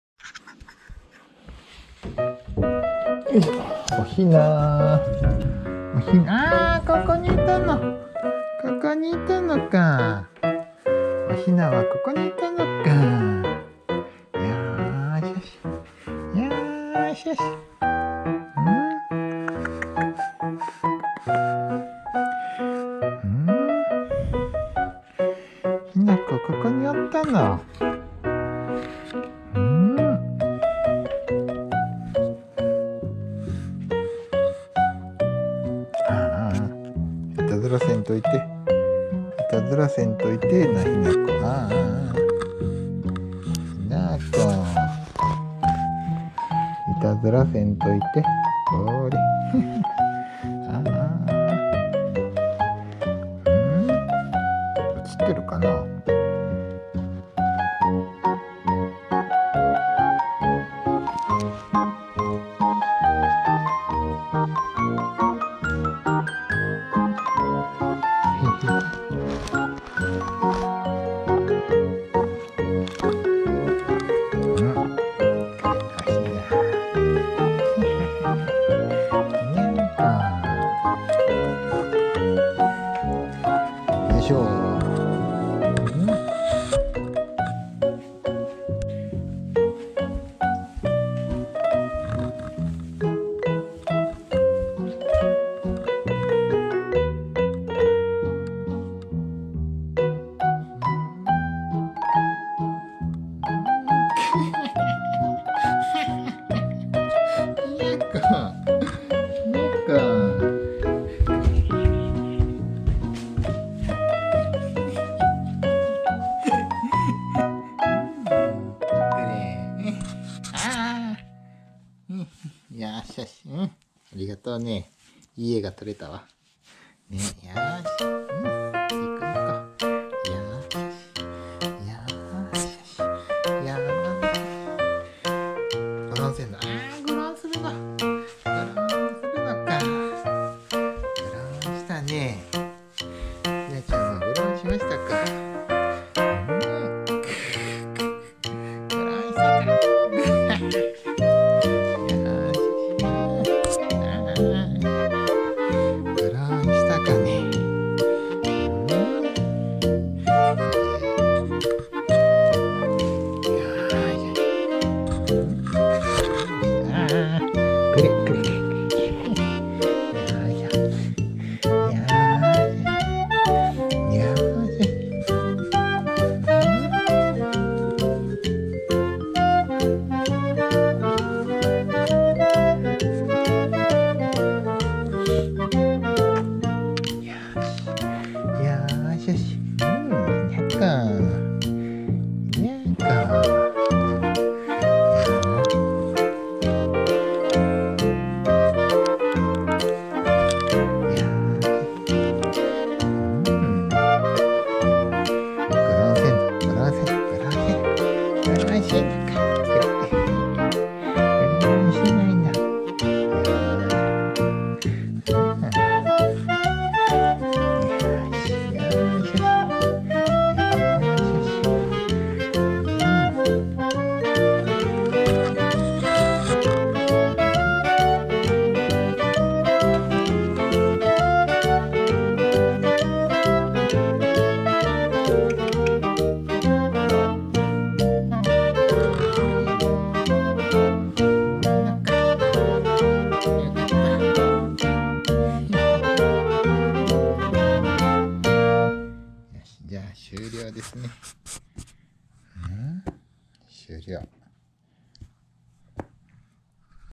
【cat】三毛猫ひなこのGoPRO動画 2022.1.25
両手がフリーになった飼い主の可愛がり♪